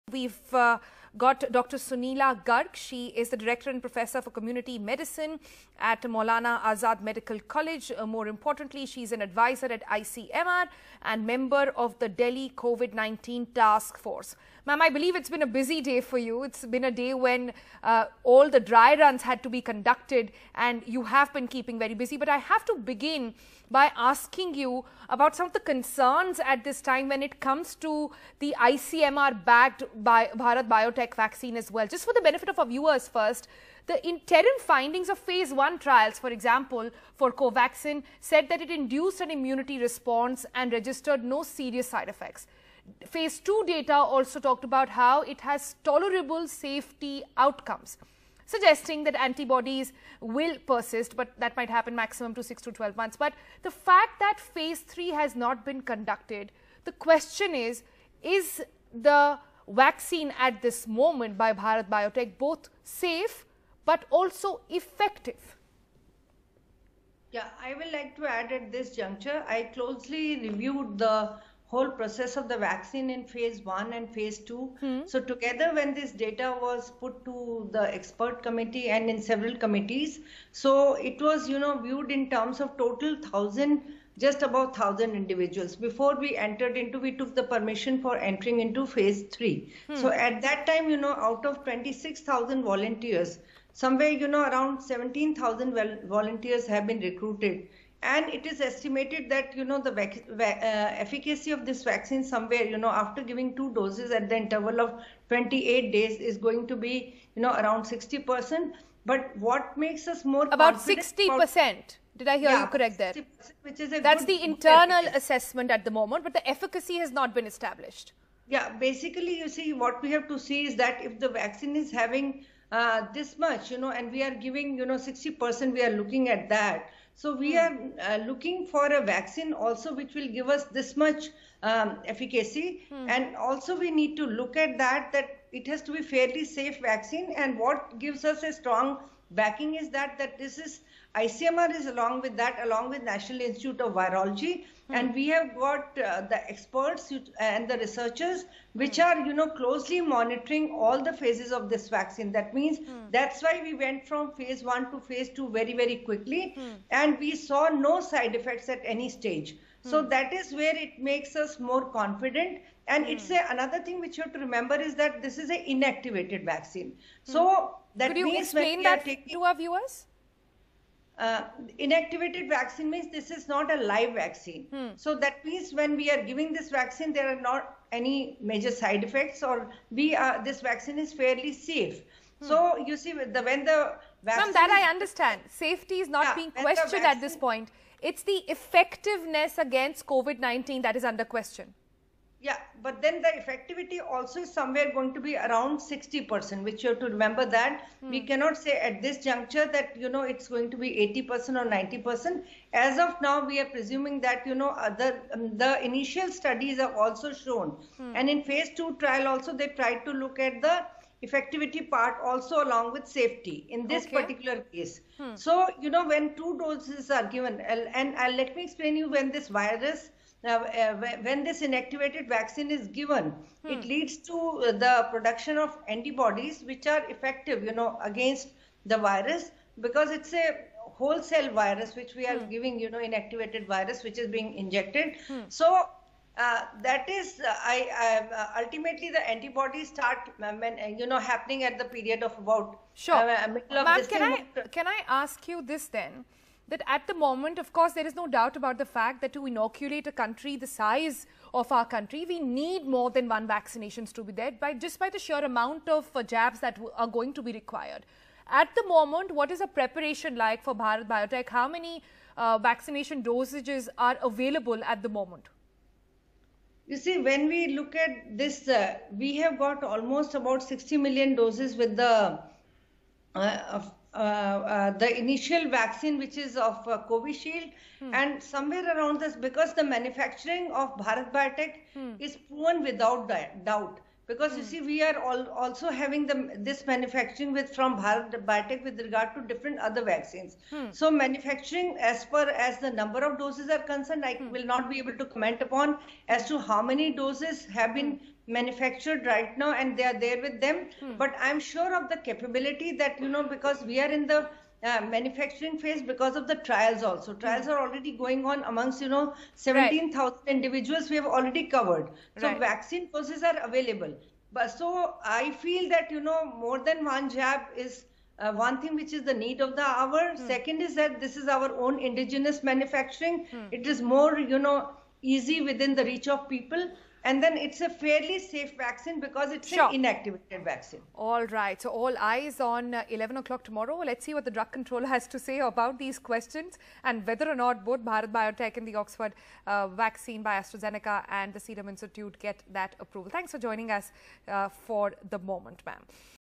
Bharat Biotech's Vaccine Cleared By Expert Panel
Covaxin, the coronavirus vaccine from Hyderabad-based Bharat Biotech, has been recommended for "restricted use in emergency situation in public interest" by a government-appointed panel, which submitted its findings to the Drugs Controller General of India on Saturday evening.